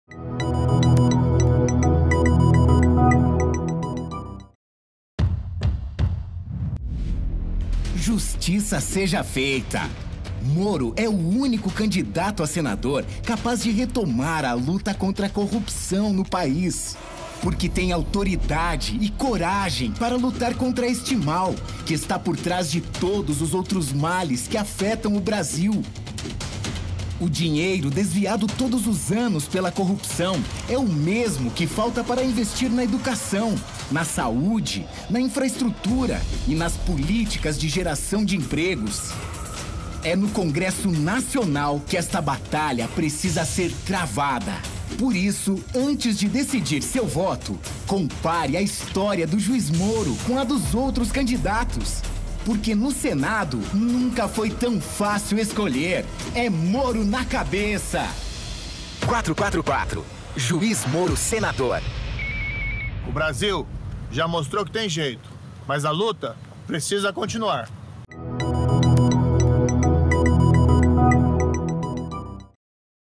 Sergio Moro (programa eleitoral 1min03seg. - TV): A luta precisa continuar (26.set.2022)
Em programa eleitoral exibido 26 de setembro de 2022, a campanha do ex-juiz e candidato ao Senado pelo Paraná Sergio Moro (União Brasil) fala que lutou contra os interesses mais poderosos.